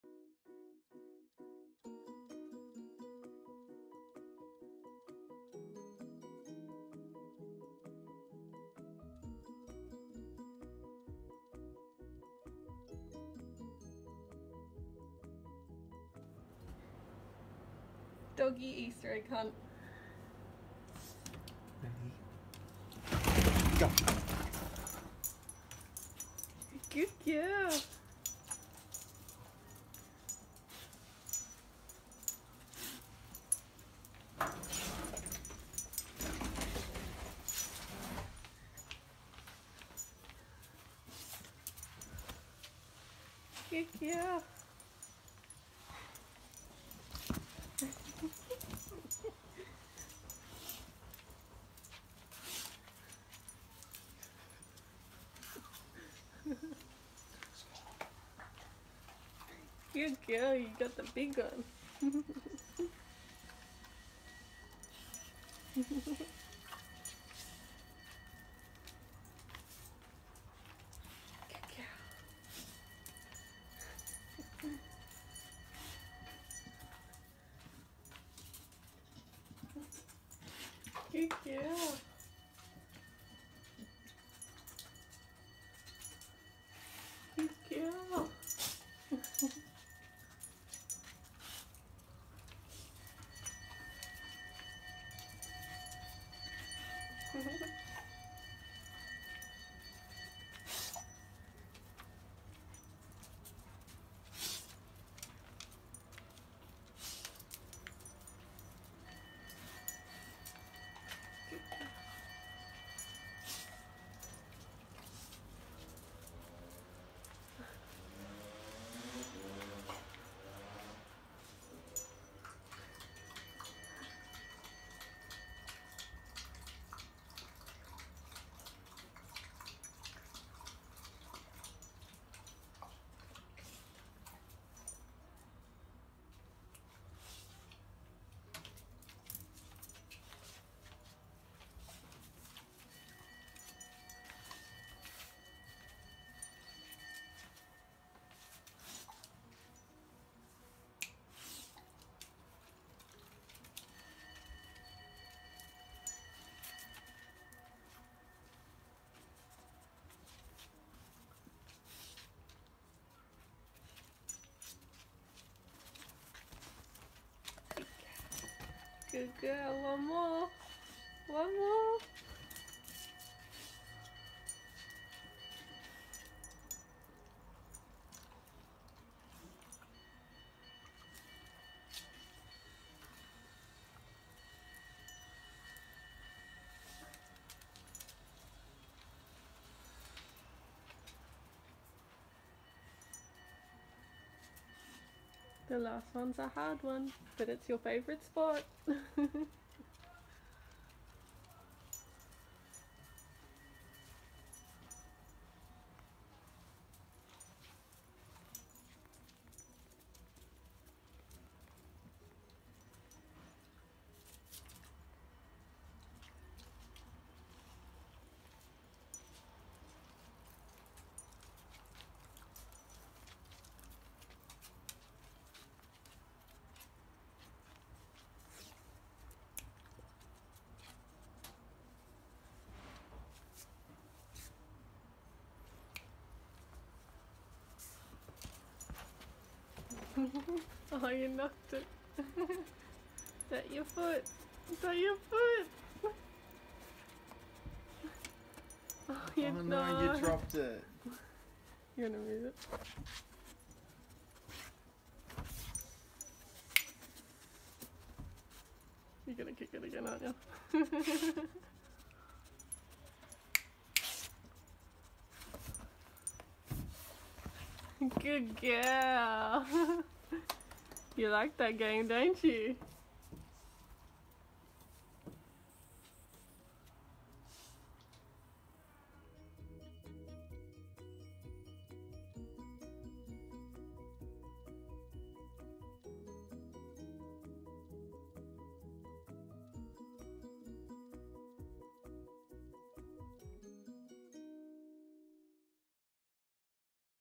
Easter Hunt for Dogs!
Happy Easter!

Today I hid treats for my dog in her long room, she was so excited to do it and then she fell fast asleep straight away!

I hope this gave you an idea to have an Easter Hunt with your pets!